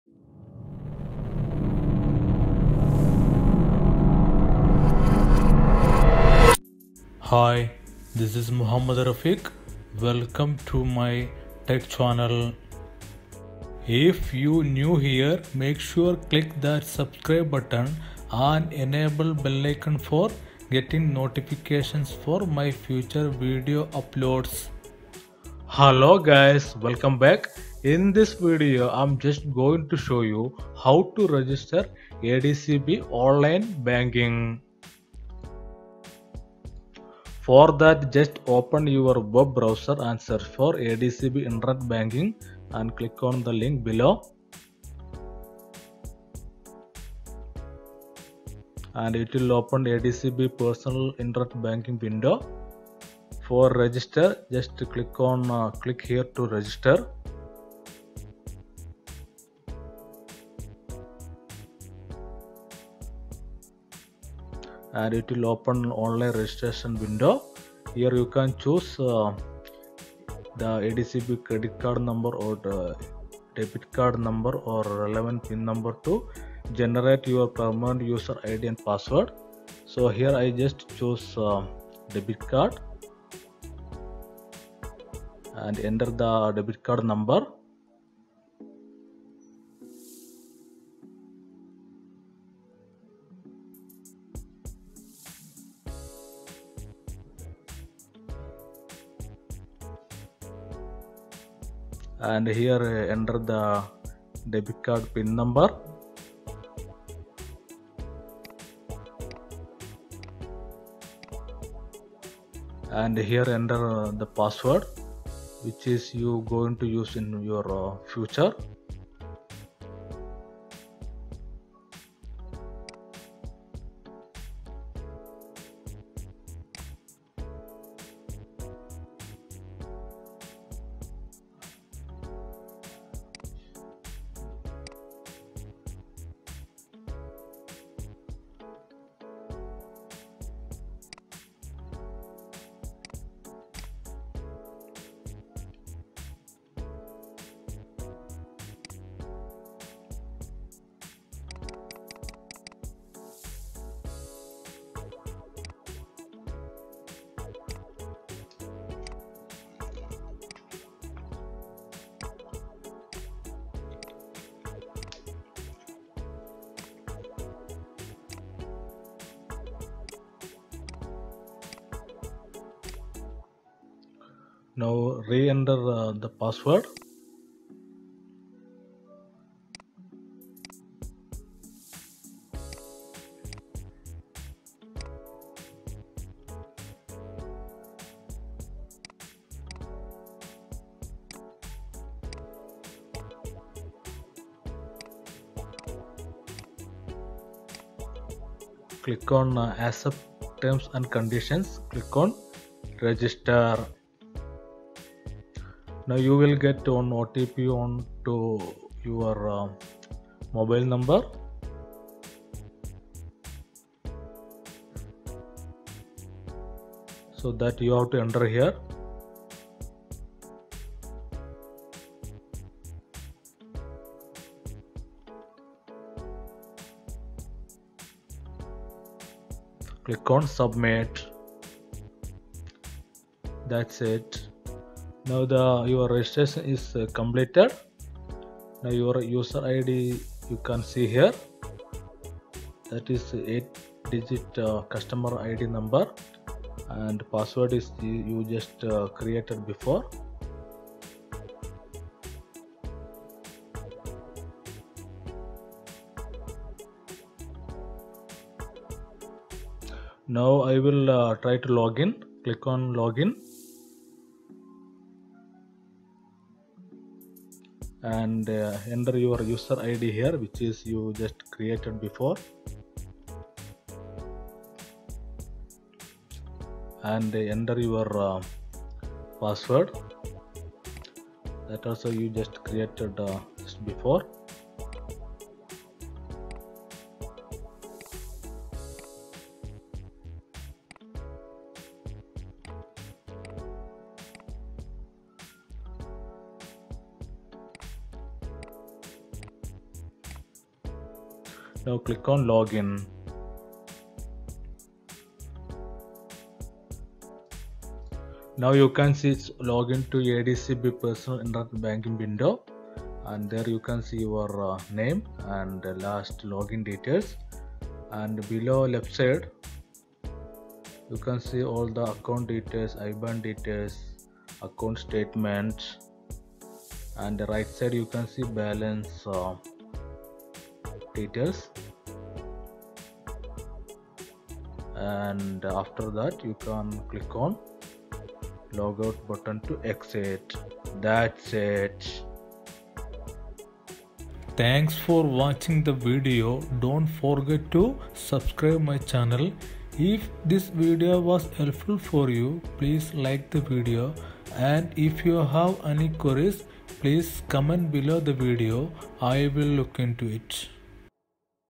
How to register ADCB Online Banking
This video is a tutorial for How to register ADCB Online Banking